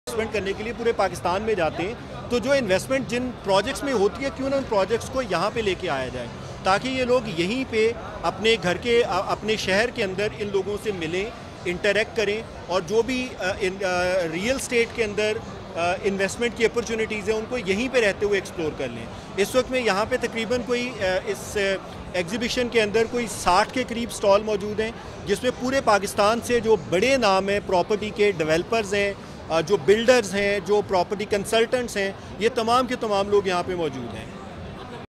Aijaz shahid talking to bol tv Peshawar property expo
Masscomm Solutions Pakistan's largest and oldest event management company